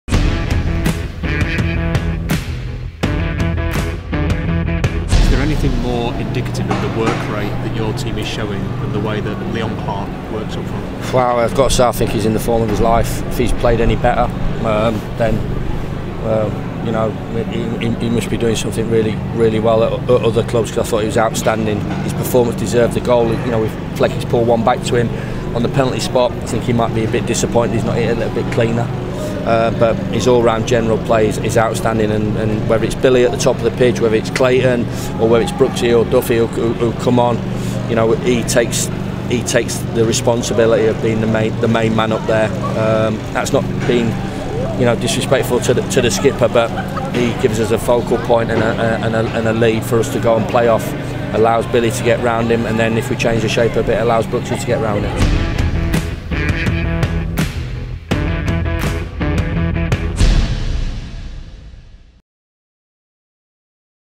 Chris Wilder on Leon Clarke
The Blades boss believes Leon Clarke is in the form of his life and more than deserved a goal at Elland Road